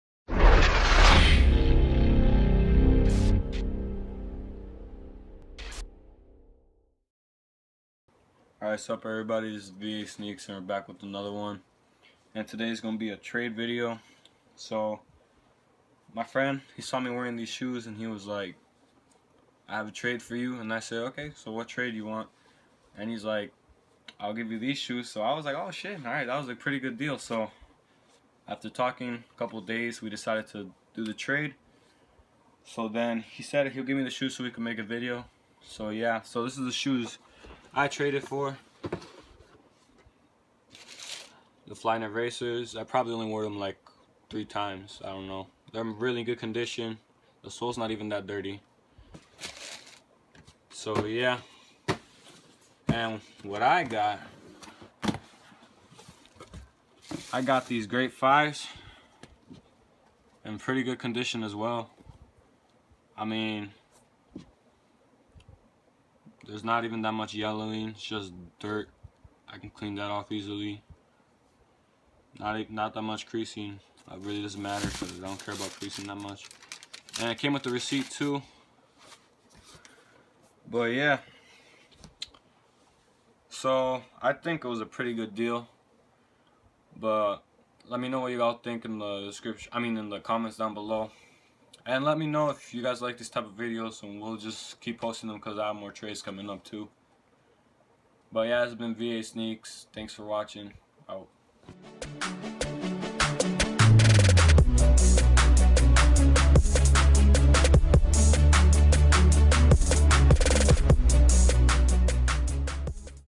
Sneaker Trade #1
Wassa this is Vasneaks back with another video, and today's video is a lil trade I did.

Comment if y'all think this was a win for me